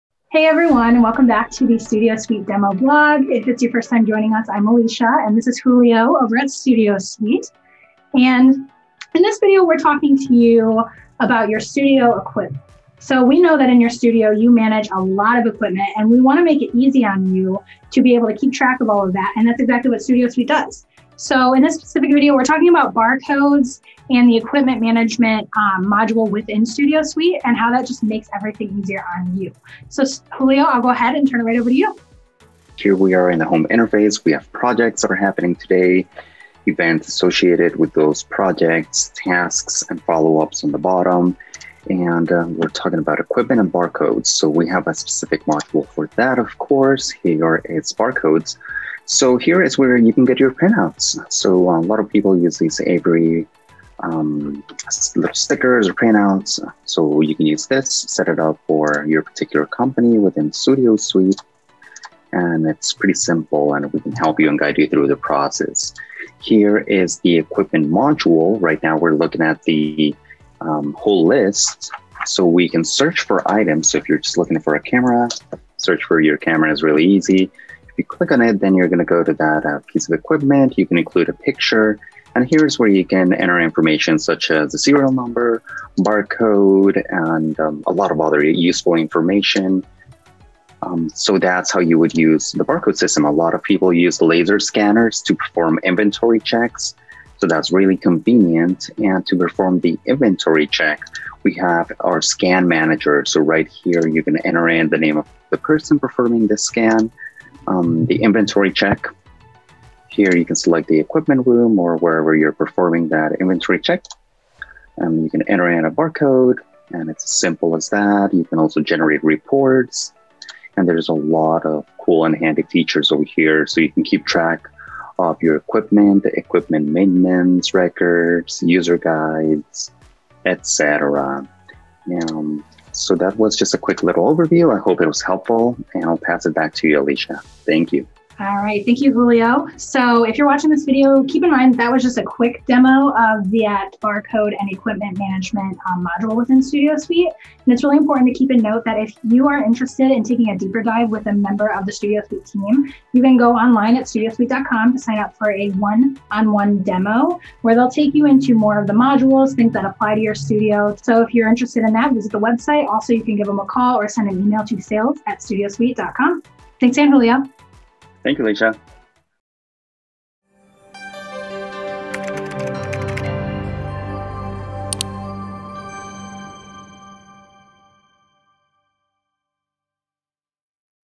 Equipment Management Module Demo
The Equipment Management Module allows studio project managers to easily track, schedule, and stock their equipment with a built-in barcode creation tool.